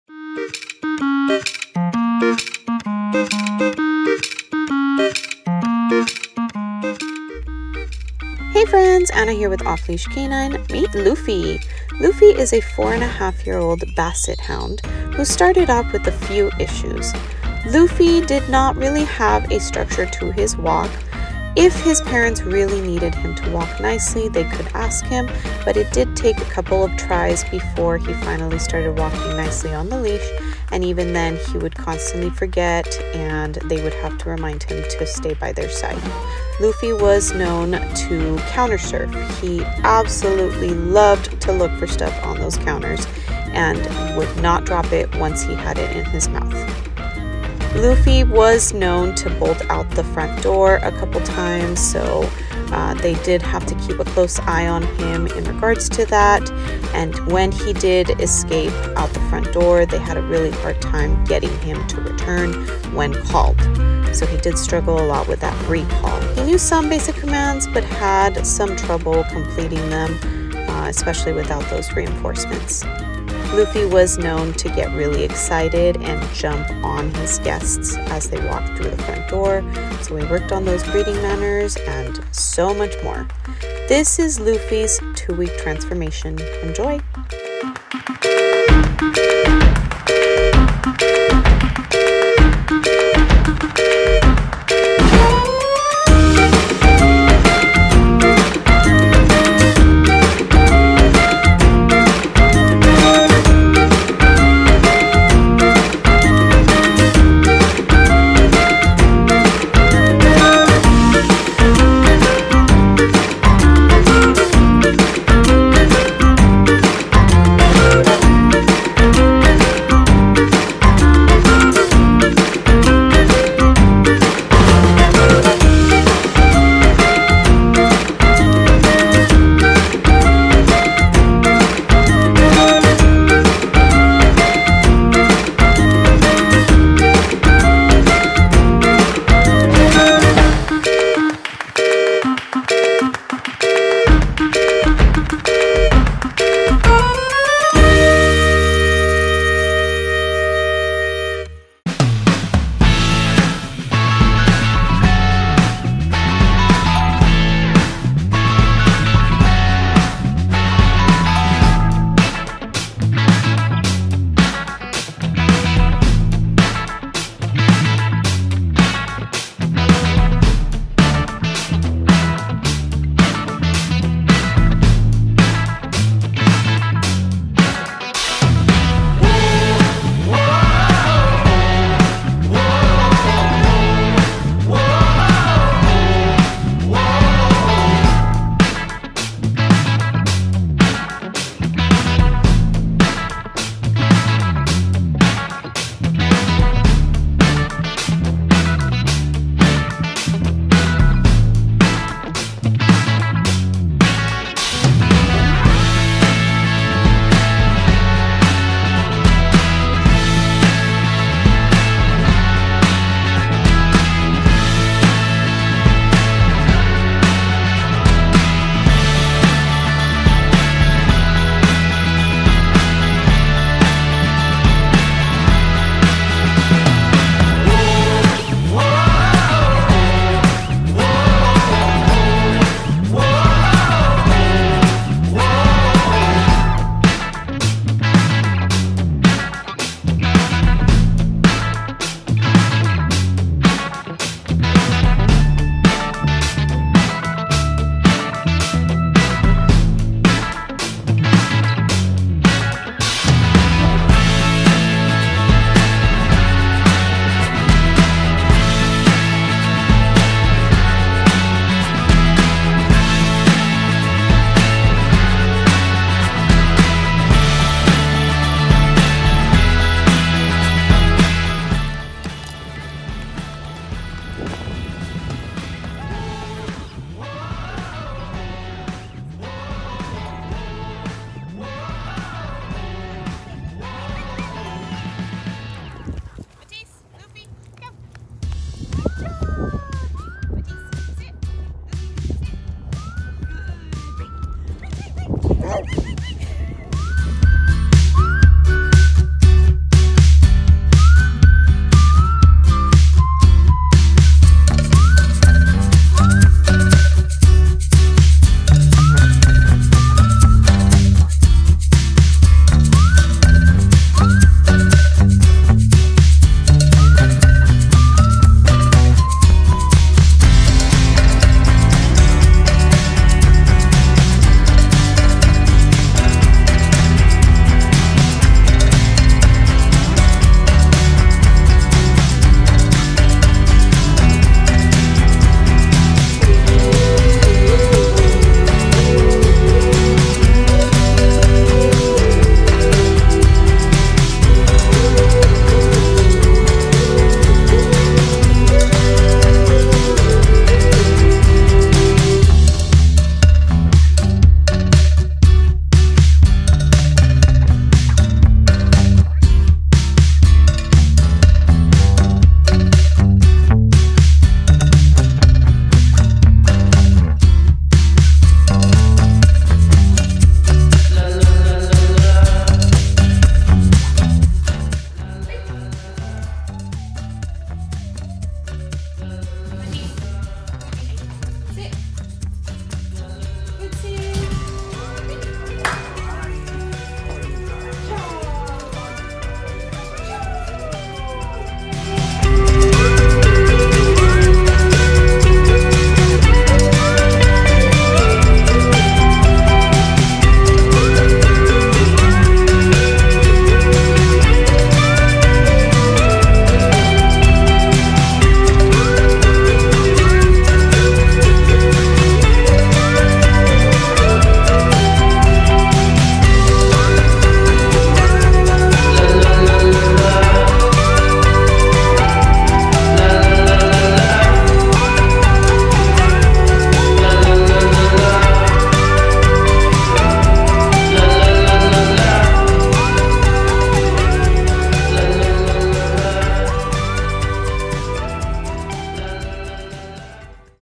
Best Basset Hound Dog Training | Salinas | Luffy
Luffy, a 4.5 year old Basset Hound, just completed dog training in Salinas. Luffy did not have a strong structure to his walk. If asked to walk nicely, he would correct himself, but it wouldn't be too long before he was pulling again. He was known to counter surf, occasionally would run out the front door and struggled to return when called. Though he knew some basic commands, he struggled to complete them without reinforcements, particularly when working with distractions. Luffy loved to greet his people by jumping on them. After his training, Luffy walked nicely next to his handlers, held his basic commands without the use of reinforcements, and learned to behave like the good pup his parents always wanted him to be.

1-Week Board and Train
2-Week Board and Train